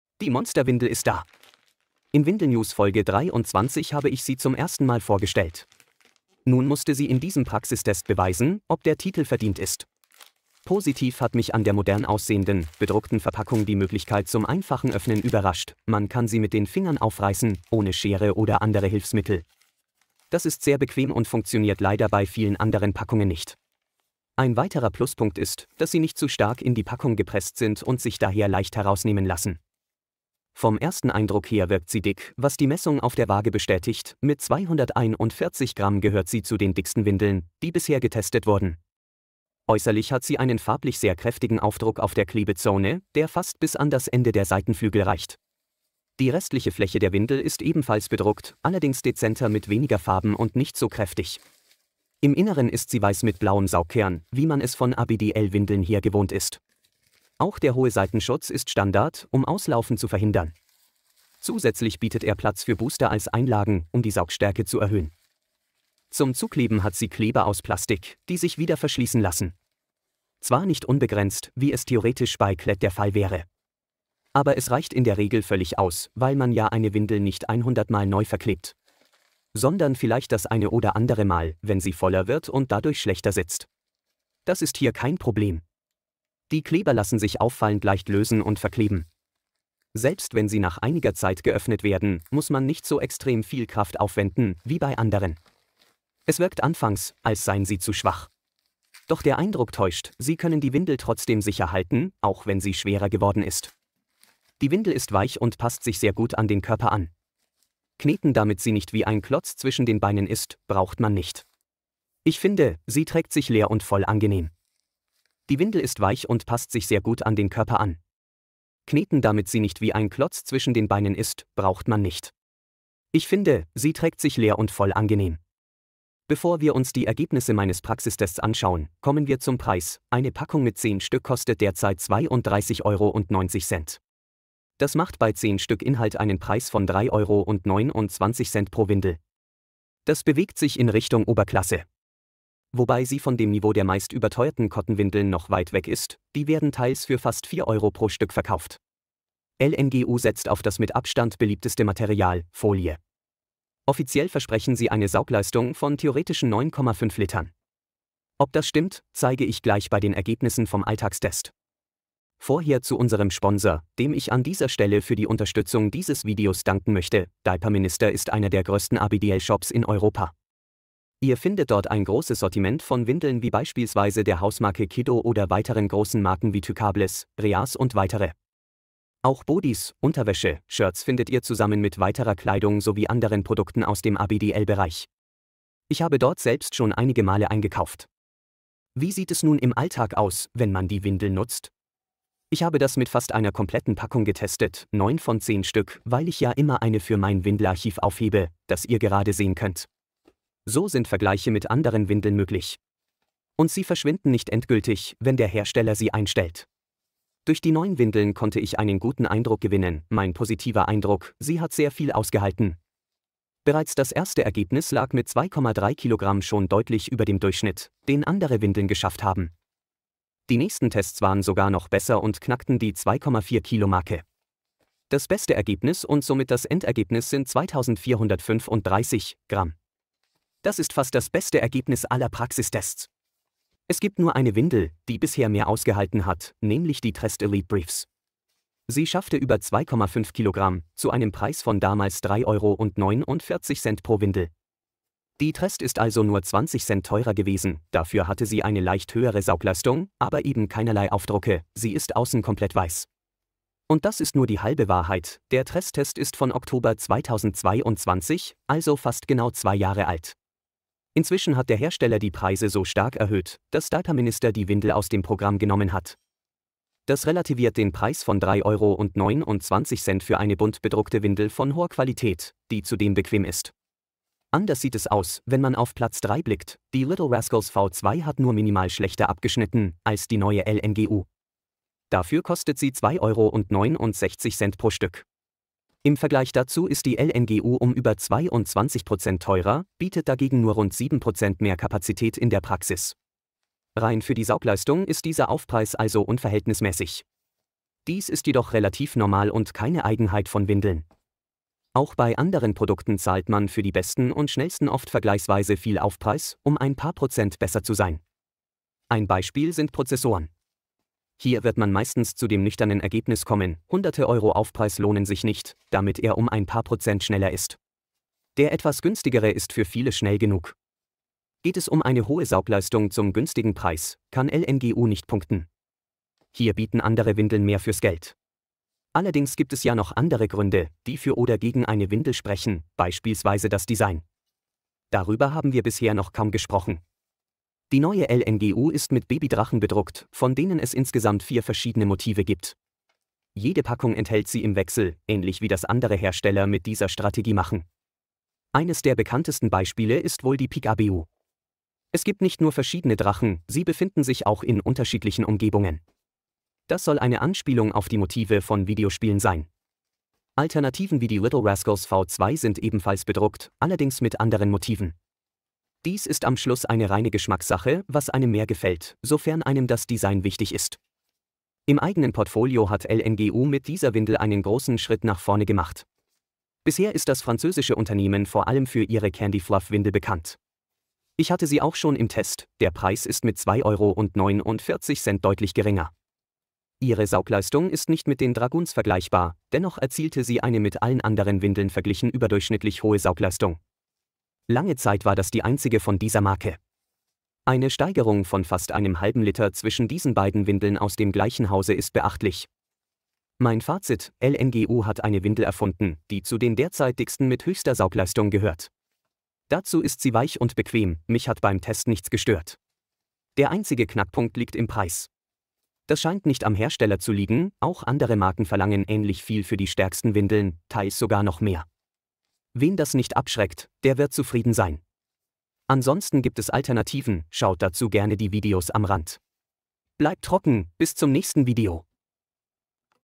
Trest kann einpacken: Die neue LNGU Dragoonz im Praxistest
LNGU hat ein wahres Saugmonster geschaffen. Wie gut ist die neue Windel des französischen Herstellers wirklich? Ich habe es im Alltag ausprobiert und zeige es euch in diesem Praxistest-Video! Außerdem vergleiche ich sie mit der Trest Elite Brief, die lange Zeit führend bei den Testergebnissen war.